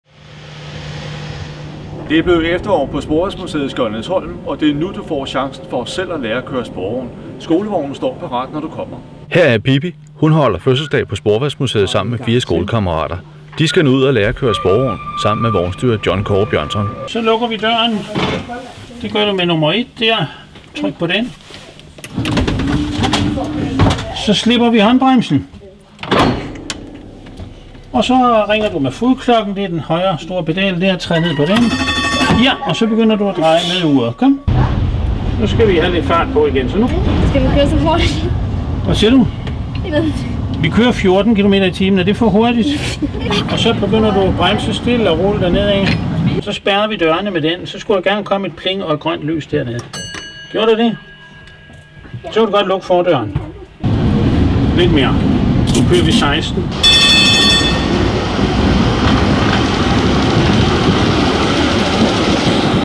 Lær at køre sporvogn
I efterårsferien kan du selv lære at køre sporvogn på Sporvejsmuseet Skjoldenæsholm